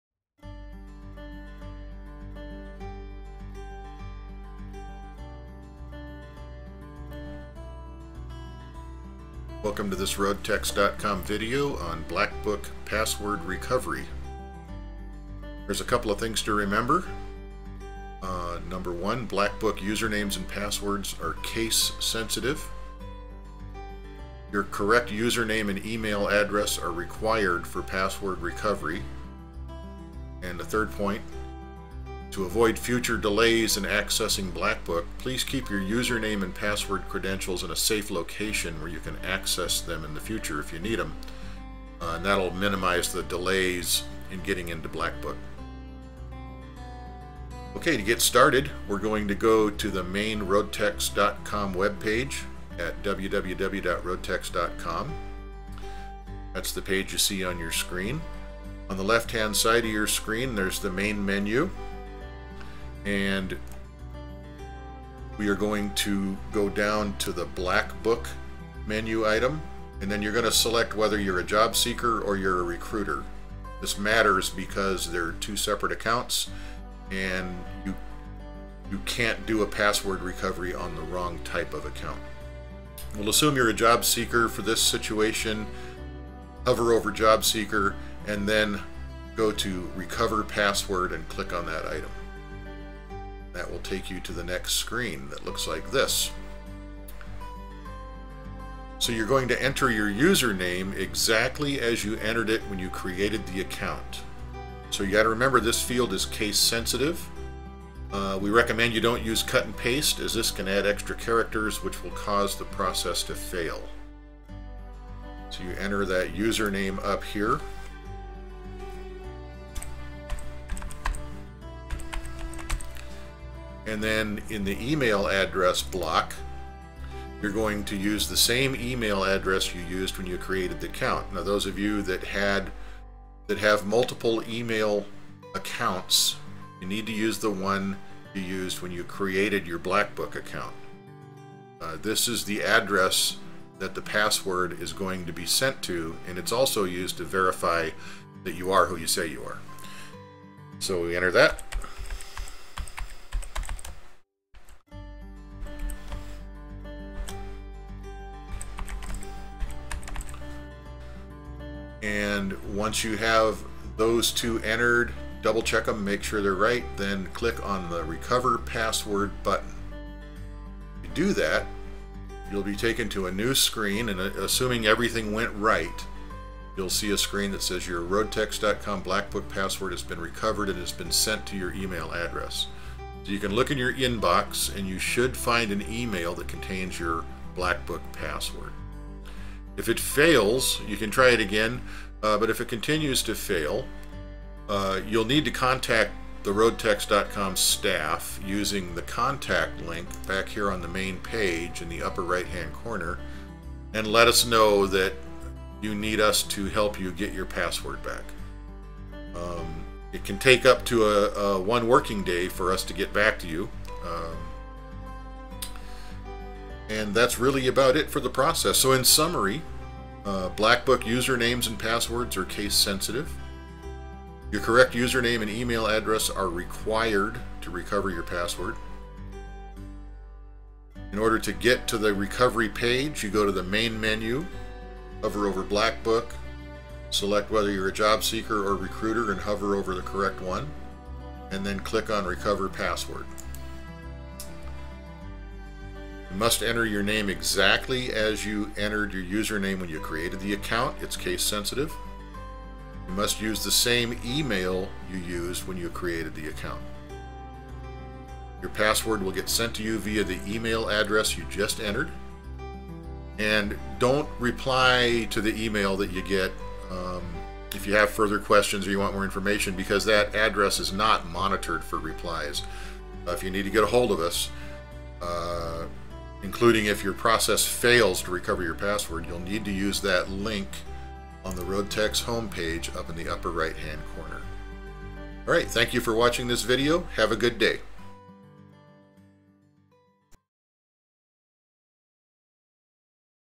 Roadtechs.com Recover Black Book Password
A short video on recovering your Job Seeker or Recruiter Black Book account password.